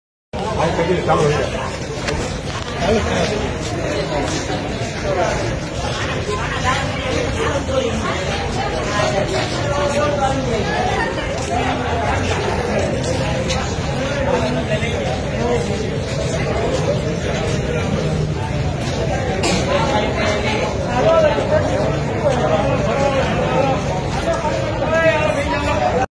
PM Modi ની મુલાકાત પૂર્વે મોડી રાત્રે Morbi ની સિવિલ હોસ્પિટલમાં હાથ ધરવામાં આવ્યું કલરકામ
PM Modi ની મુલાકાત પૂર્વે મોડી રાત્રે Morbi ની સિવિલ હોસ્પિટલમાં હાથ ધરવામાં આવ્યું કલરકામ | Mumbai samachar